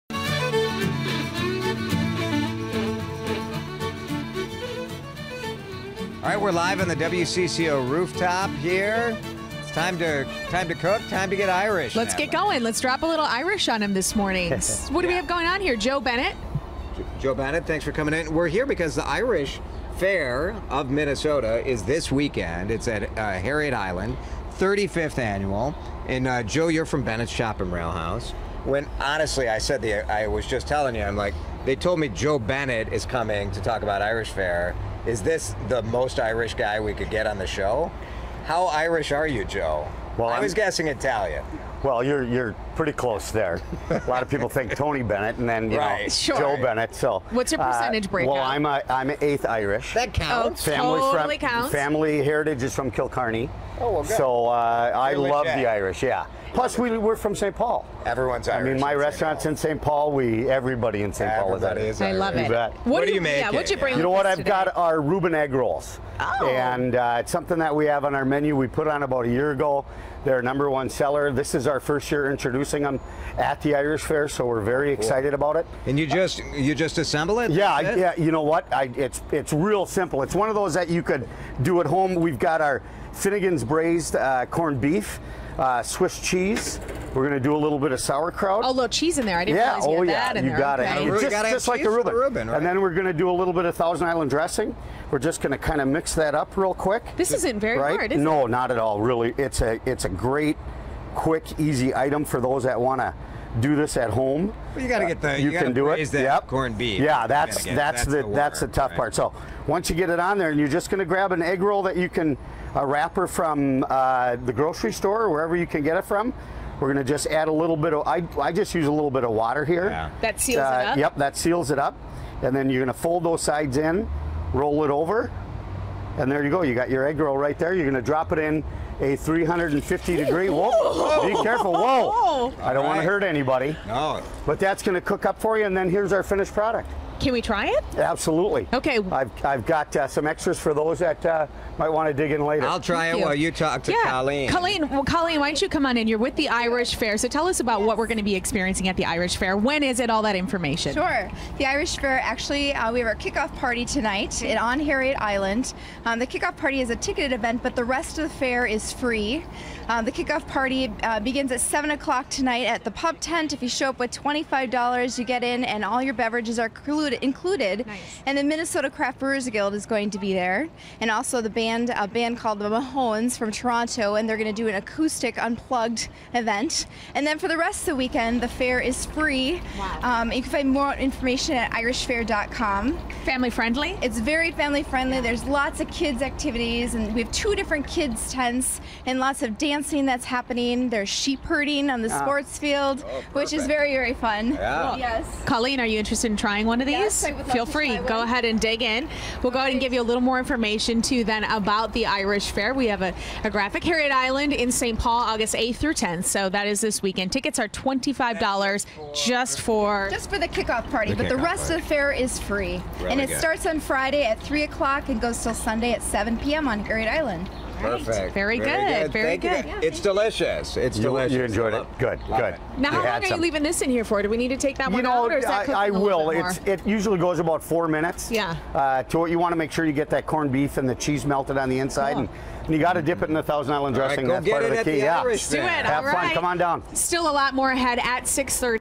Cooking On The Rooftop: A Taste Of The Irish Fair Of MN
Joining us is Chef Joe Bennett from Bennett's Chop &amp; Rail House to give us a sample of what you can try this weekend. (3:59) WCCO This Morning – Aug. 7, 2014